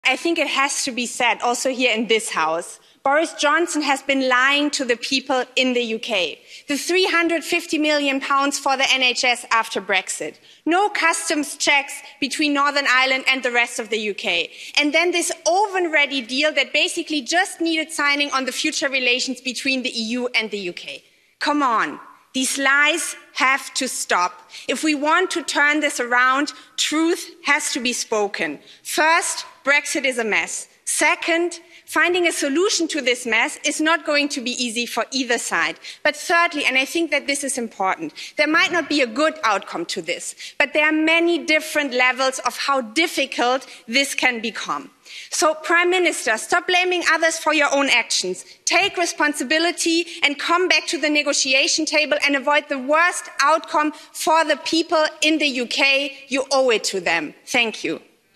German MEP's scathing attack on Boris Johnson over Brexit | LBC
A German MEP has launched a scathing attack on Boris Johnson, telling him to 'take responsibility' for Brexit and not 'lie to the people of the UK'